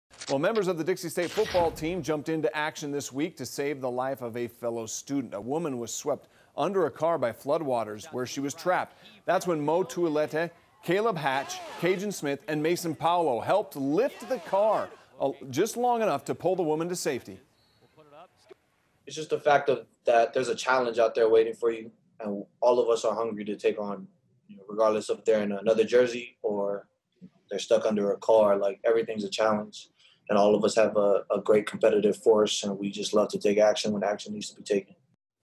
Dixie State Football Team Lifts Car, Saves Woman’s Life During St. George Flooding
Members of the Dixie State football team saved another life this weekend during flooding that happened in St. George on Sunday.